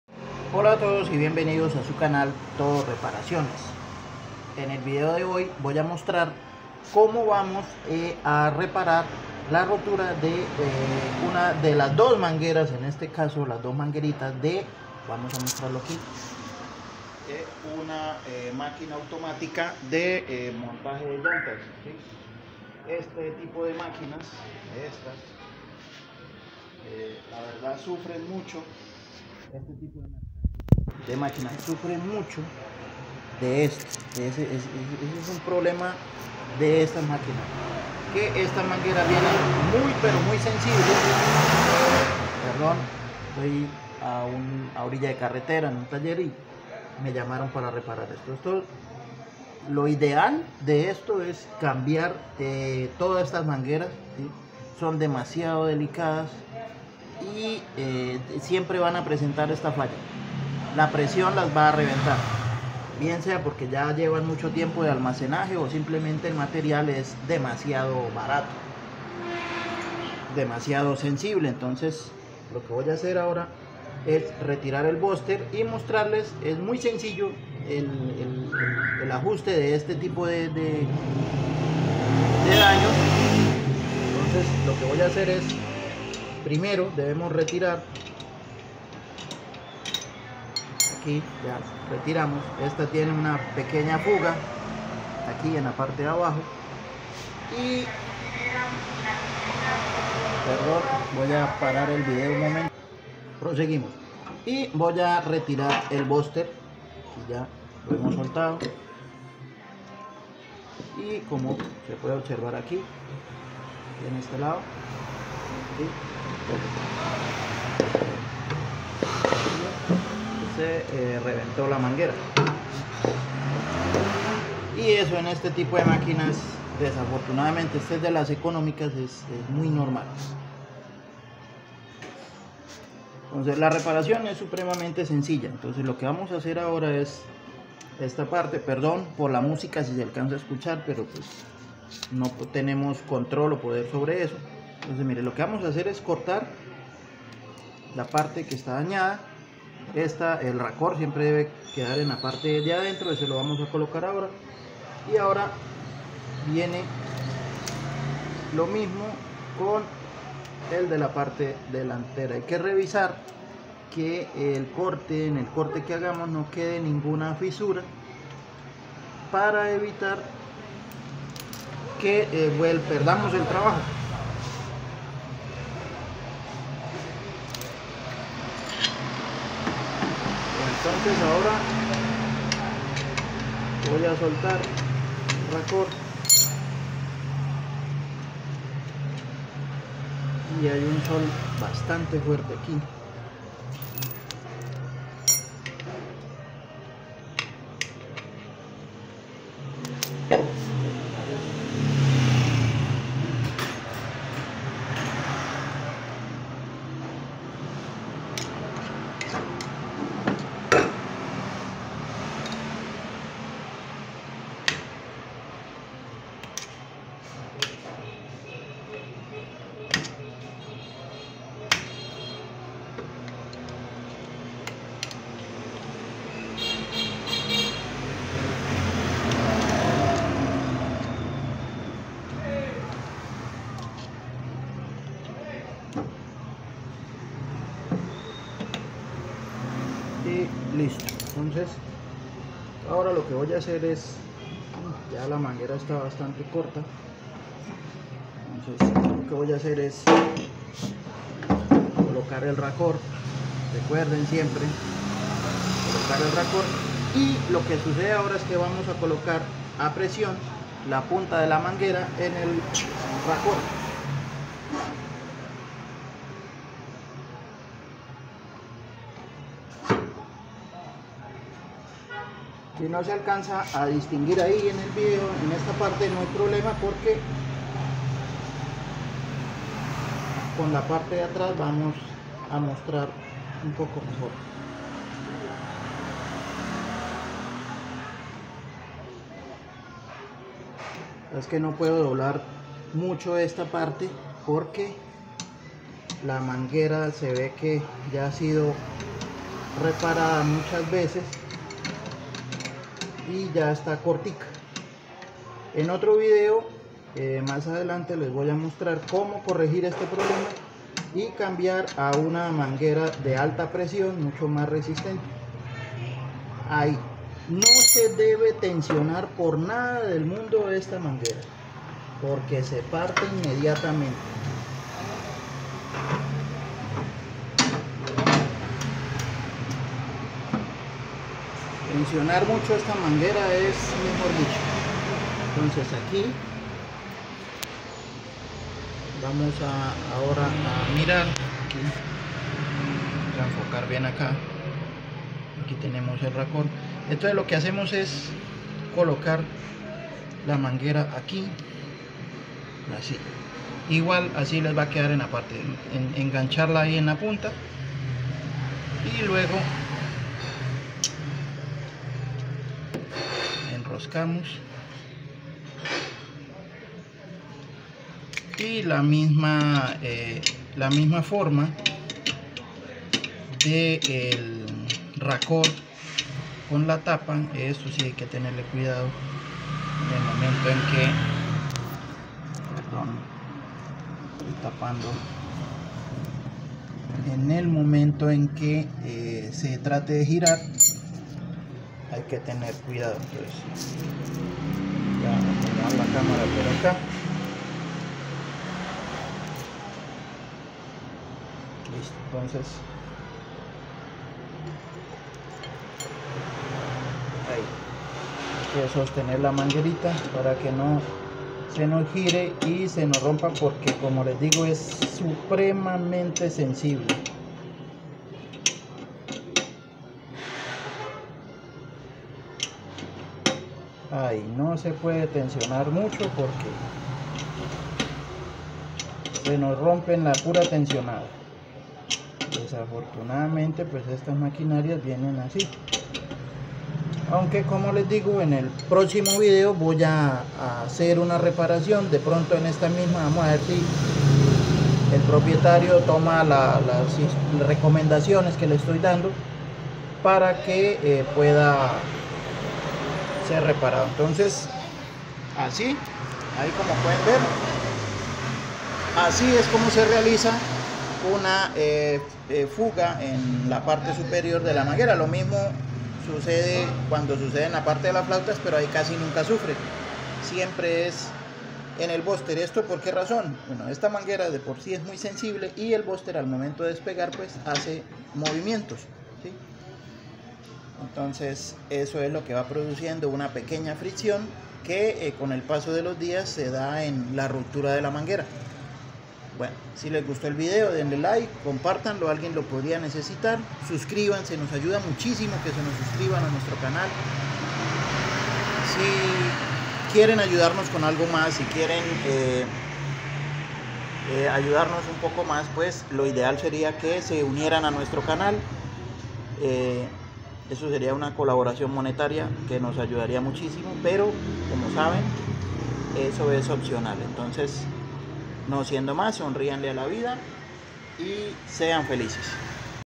CÓMO CAMBIAR LAS MANGUERAS DE UN BOOZTER máquina MONTALLANTAS. HOW TO CHANGE THE HOSES ON A BOOZTER
En este video te enseño a reparar cuando se rompen las mangueras de una máquina de montaje de llantas.